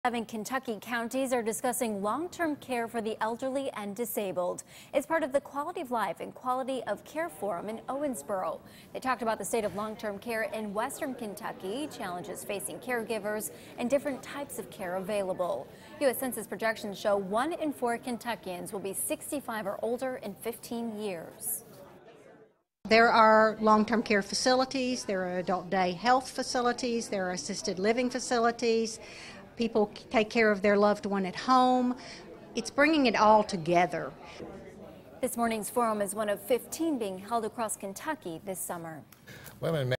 GRADD -- Long Term Care Forum -- Eyewitness News (8/12/14)
GRADD hosted the "Quality of Life and Quality of Care Community Forum" at Logsdon Community Center on August 12. Agency representatives, caregivers and interested citizens discussed the future of long term care in our region.